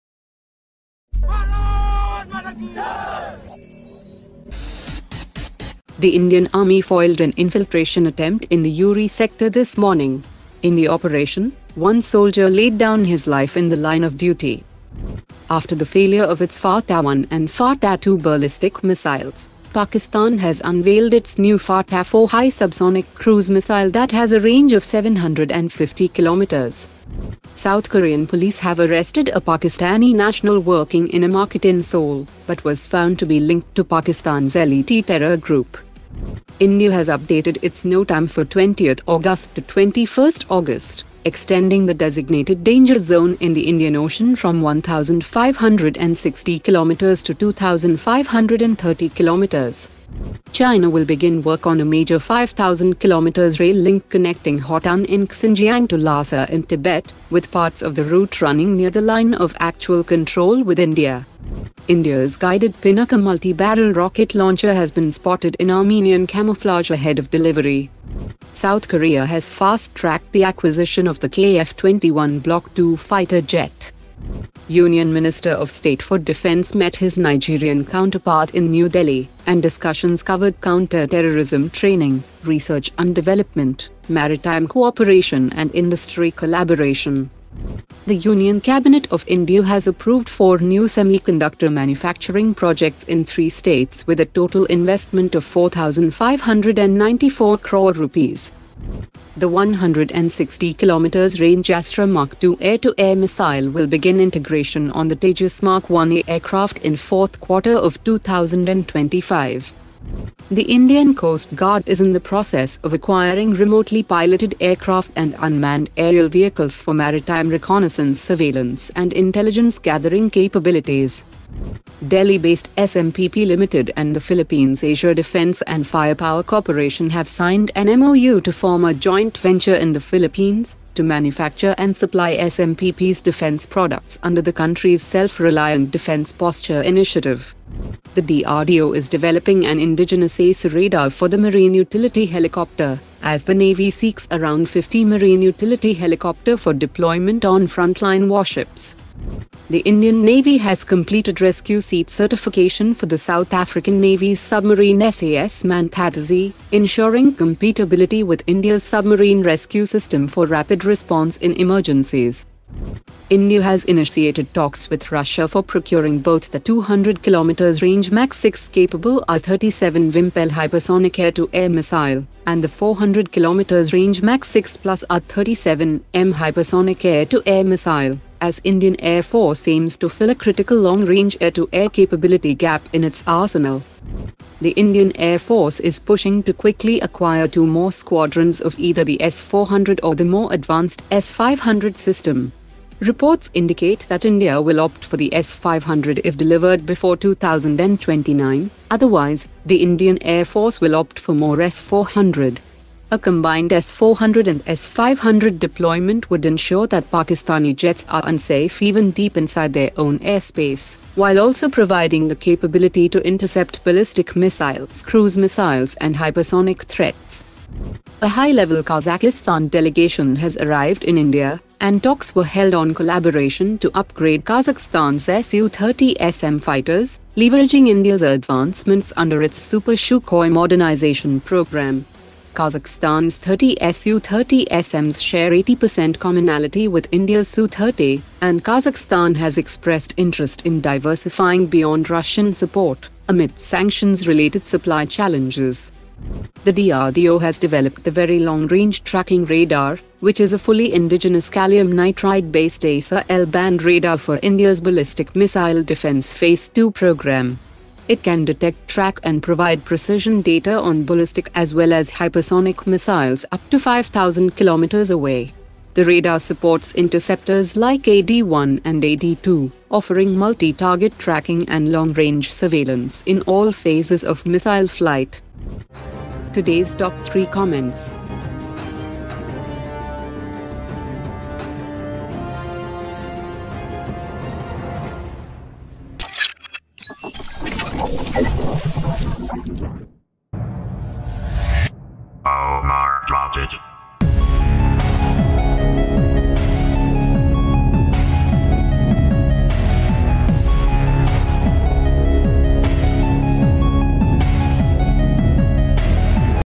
Indian Defence Updates : S-500 Order,Hypersonic Missile Deal,Super Sukhoi Export,BMD Phase-2 Radar
1. Pakistani Terro-ris-ts Infiltration Bid Foiled in Uri
2. Pakistan Unveils New Fatah-4 Cruise Missile
3. Pakistani LeT Te-rro-rist Arrested in South Korea’s Seoul
4. India Extends Missile Test Zone in Indian Ocean to 2530 Km
5. China to build 5000 Km Xinjiang Tibet Rail Link Near LAC with India
6. Guided Pinaka Launcher spotted in Armenian Camoflage
7. South Korea to Fast-Track KF-21 Block-2 Fighter Jet
8. India Nigeria Discuss Counter Ter-ror & Defence Cooperation
9. Cabinet Approves 4 Semi Conductor Plants Entailing 4594 Cr Investment in 3 states : Odisha, Punjab and Andhra Pradesh
10. Astra Mk-II Missile Integration on Tejas MK1A Set for Q4 of 2025
11. Indian Coast Guard to Induct UAV's for Enhanced Coastal Surveillance
12. India expands Southeast Asia defence footprint with new joint venture in Philippines
13. DRDO developing AESA Radar for Naval Utility Helicopter
14. Indian Navy Certifies South African Submarine for Rescue Operations
15. India in talks to procure 2 Hypersonic BVRAAM from Russia : R-37 Vympel & R-37M
16. IAF to seek two S-500 Batteries if delivered by 2029
17. Kazakhstan seeks India's Expertise for Su-30 Upgrades
18. DRDO Unveils VLRR : GaN-Based Very Long Range Radar for BMD Phase-II